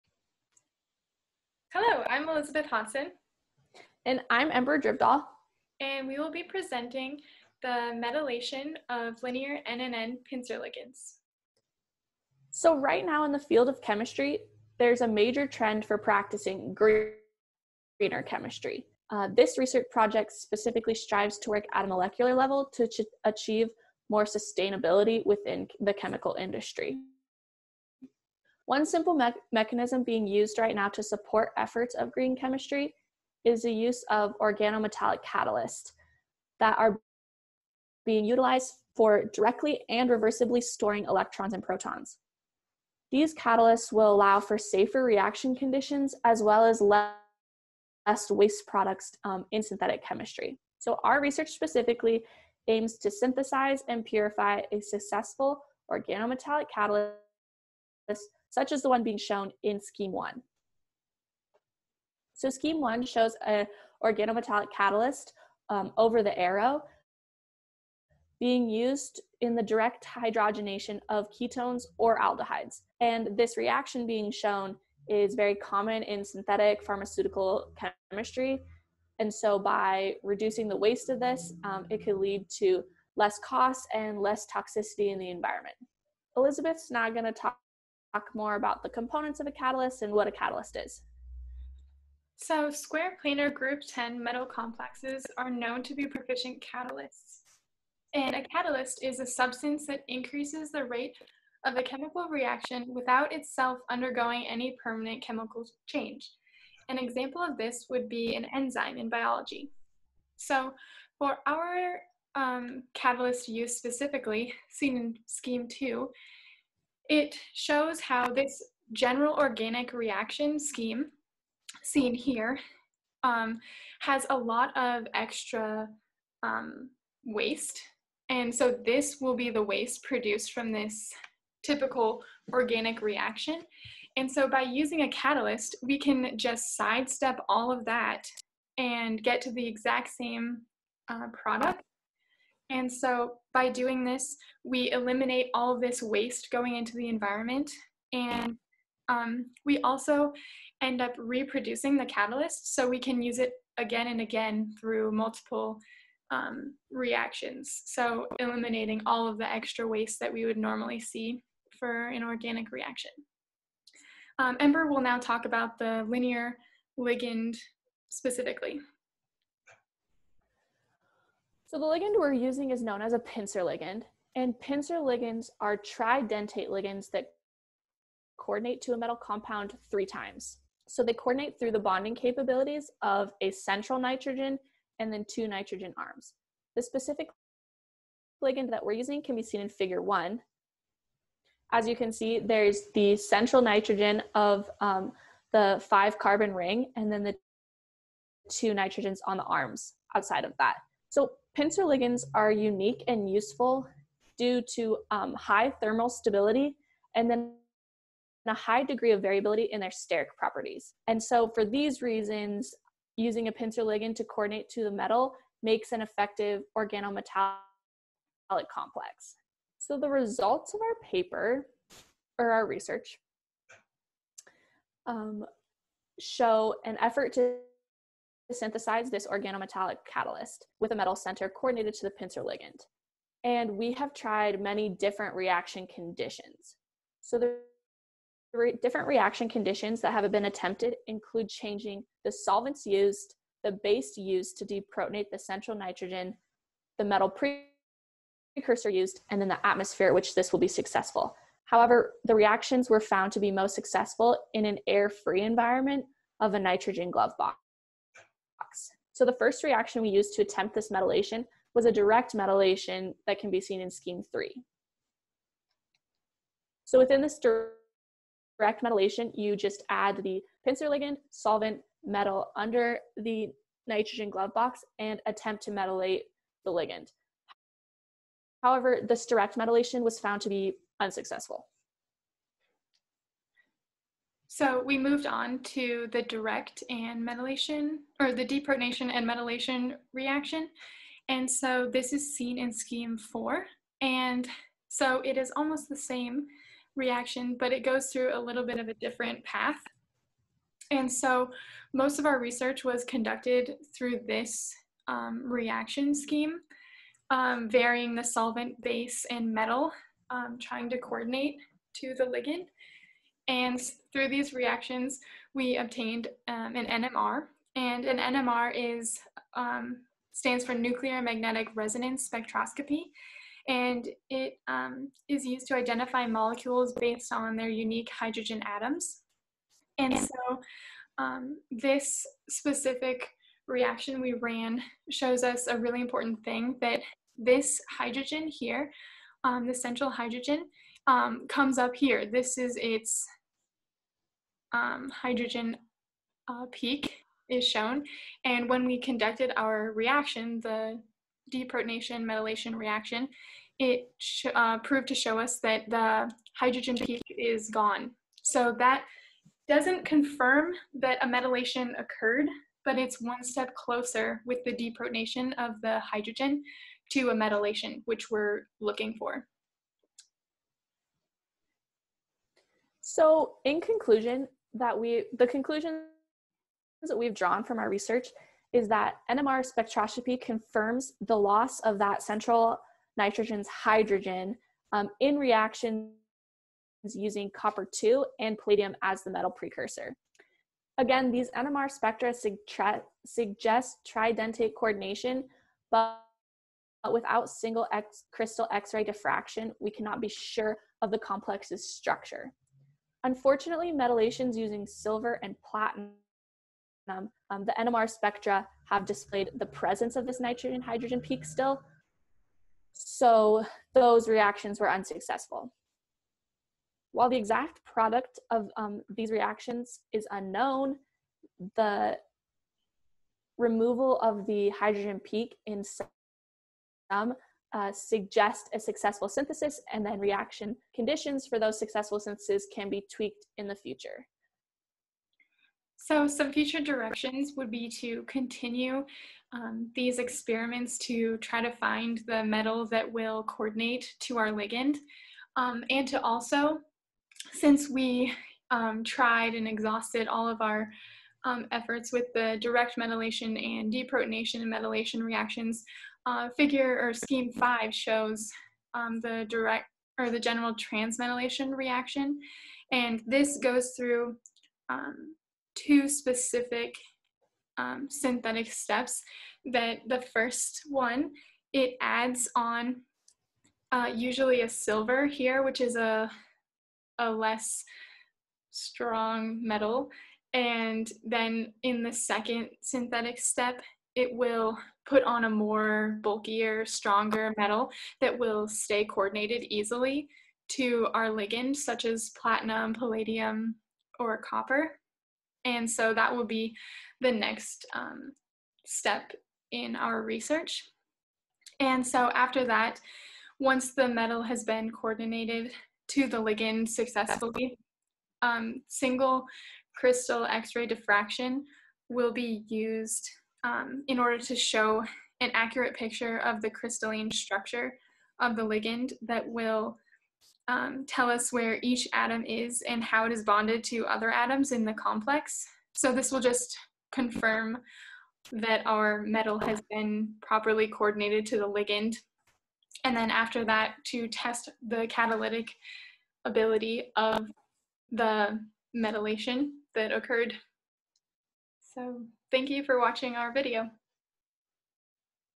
Metalation of Linear NNN Pincer Ligands
Elizabeth Hodgson and Ember Drivdahl, Carroll College

A major trend of the chemical industry today is the practice of “greener” chemistry, including the reduction of waste. One simple mechanism to support the efforts of green chemistry is the use of organometallic catalysts. The purpose of this work is to coordinate the tridentate pincer ligand 2,5-bis(diethylaminomethyl)pyrrole (py-NNN-Et) to a metal center such as nickel, palladium, or ruthenium. Thus far we have strategically varied reaction conditions including metal precursor, solvent, and use of base in an attempt to synthesize products of the form M(pyr-NNN-Et)Cl (M=Ni, Pd, Ru). Crude reaction products have been analyzed primarily by multinuclear NMR spectroscopy (1H, 13C, 31P), and purification via recrystallization is underway. Next, single crystal X-ray diffraction will be performed to confirm the coordination mode of the ligand and molecular geometry of the product. Square planar group 10 metal complexes are known to be proficient catalysts for the direct hydrogenation of cyclic ketones to form alcohols, which would be a specific reaction, amongst other common organic chemistry transformations, in which our resulting catalysts would be evaluated. Ultimately, this work will impact the advancement of an eco-friendly field of chemistry.